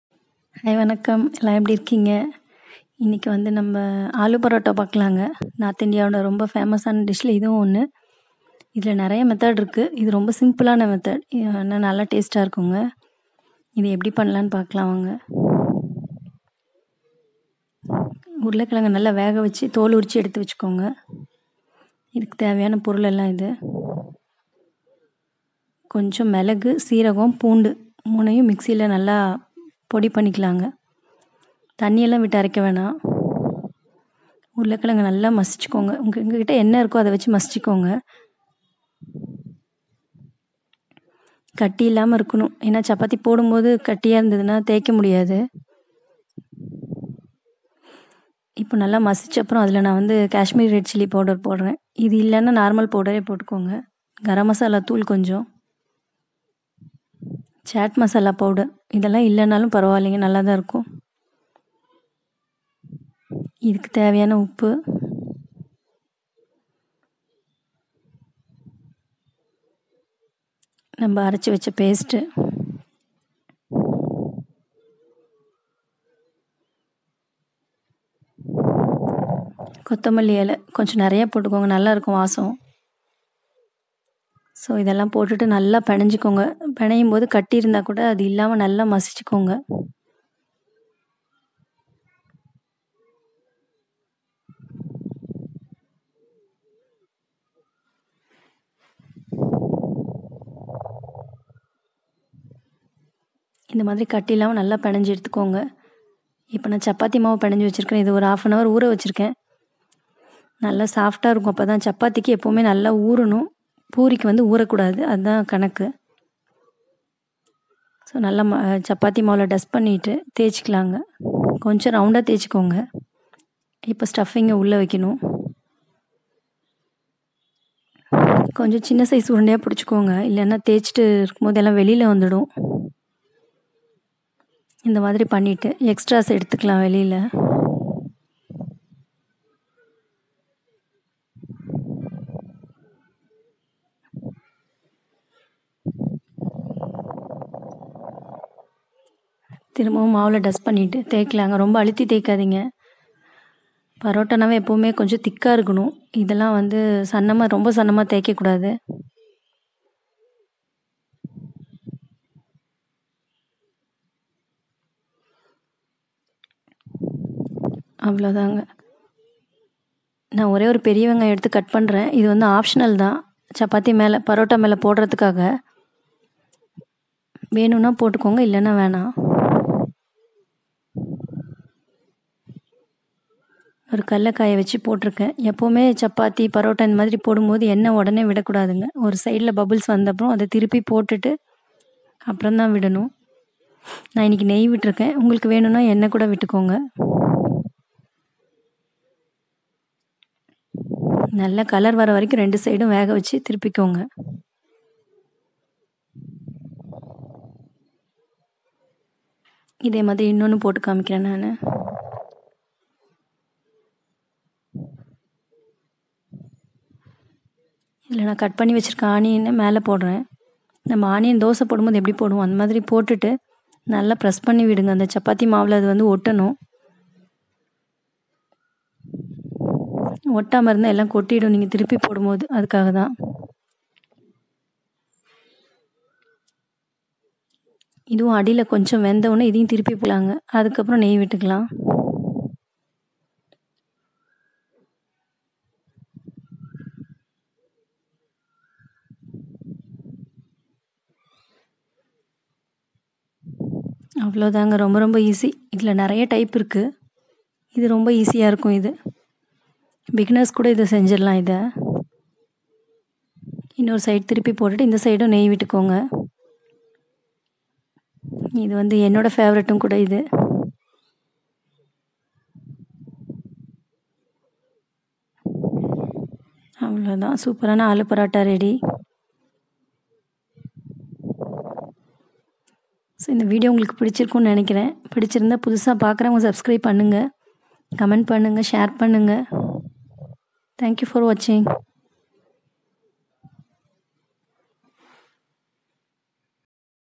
உருளை கிழங்கு பரோட்டா செய்து அசத்துங்க|| Aloo Paratha
Potato - 1/2 kg
Garlic - 10
Chilli powder - 1 spoon
Chat masala - 1/4 spoon
Garam masala - 1/4 spoon
Coriander leaves
Chapathi dough
Ghee
Oil